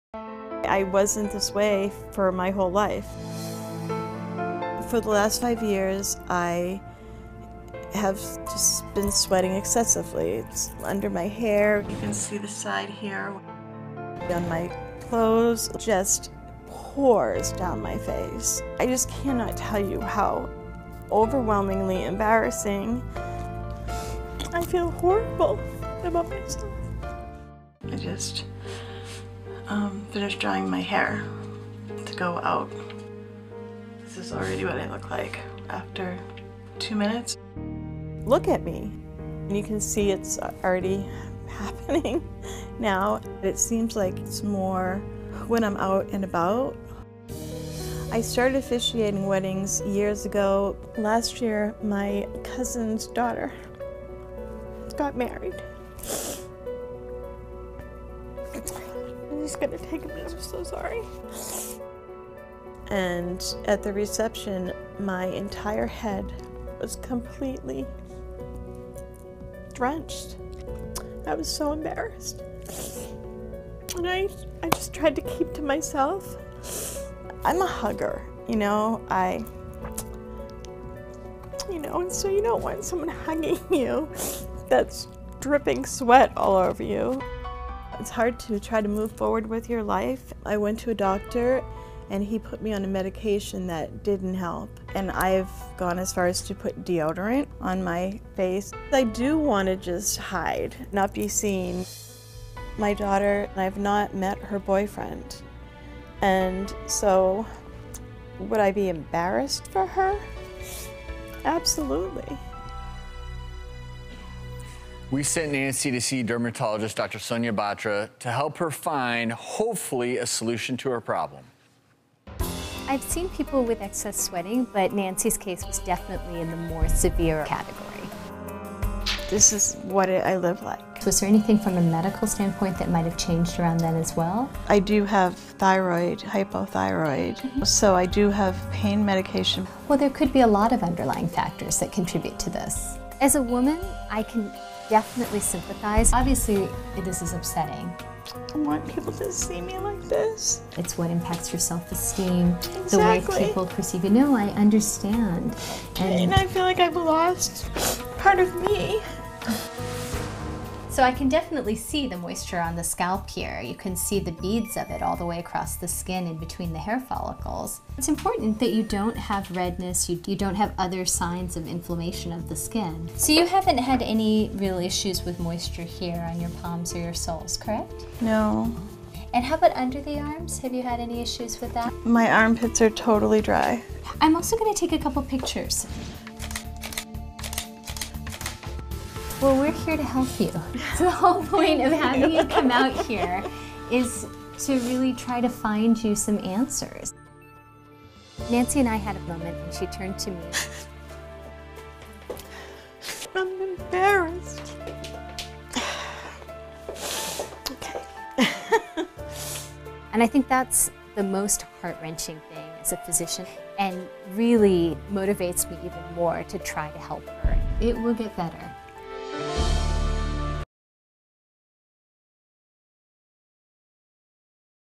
Woman Who Cant Stop Sweating
Dr. Sonia Batra meets with a patient with severe craniofacial hyperhidrosis. Can she help?